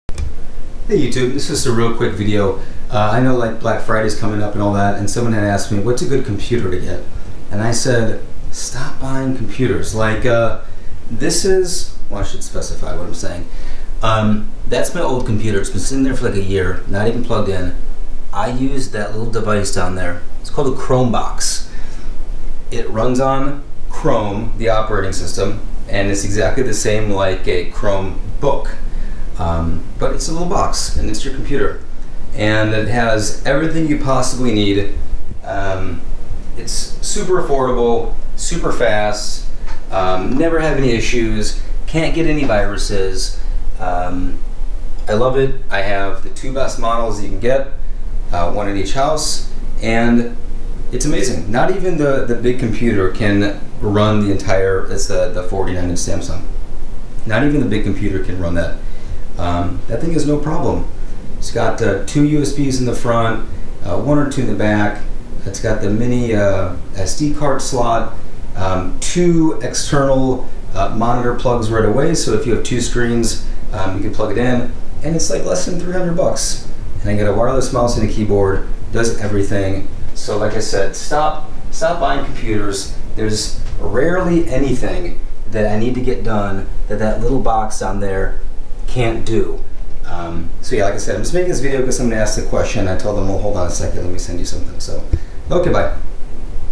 Don't buy a regular computer - a Chromebox is all you need
To Dave who asked what computer to buy - For 2 years I've been using chromeboxes and they do 99% of what I need to do. They are small, affordable and can't get viruses. Save your money!
In this video I have the ASUS Chromebox 3 CHROMEBOX3-NC356U and at my other house I have the Acer CXI2-4GNKM